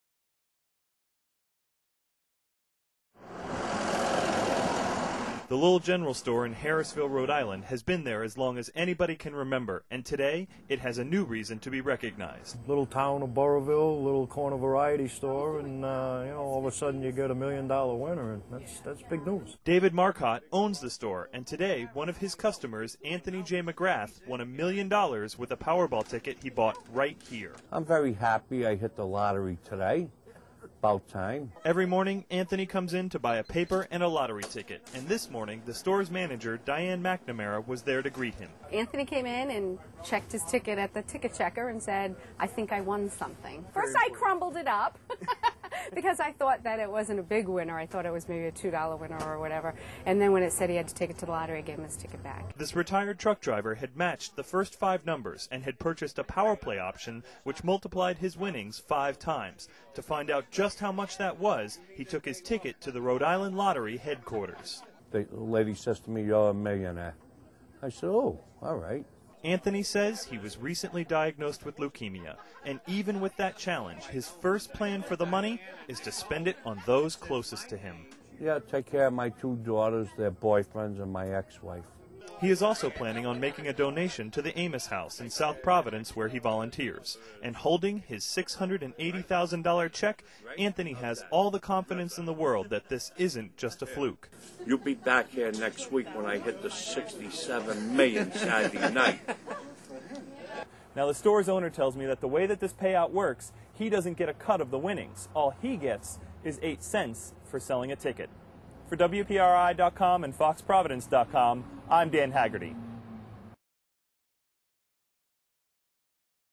Dan Haggerty Reports:Burlville Man Wins It Big
A local man from Burriville, RI win $1 million at Powerball.